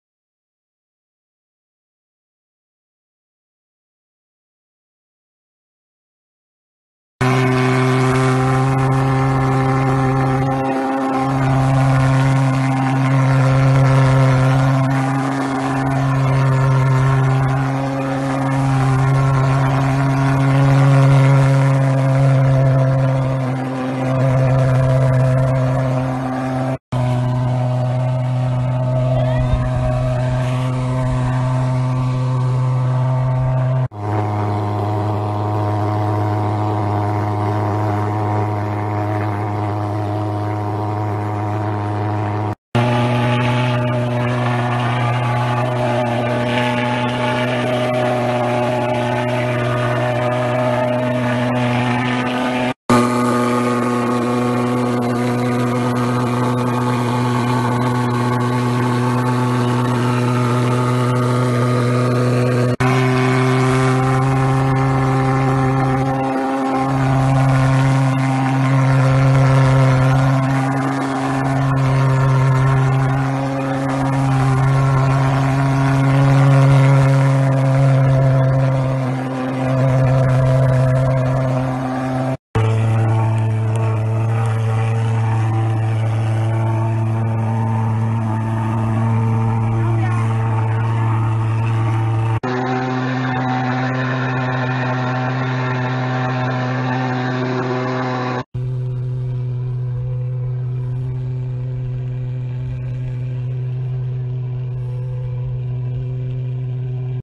Fogger Machine in India By Bharat Agritech
Presenting the best quality Thermal fogging machine with a back pack tank reservoir.Very easy to operate fogger machine.This machine is useful for all farmers as well as for pest control service providers.This Very high quality fogging machine is available at very competitive price.
A mini fogger is a device that creates a fog, typically containing an insecticide for killing insects and other arthropods.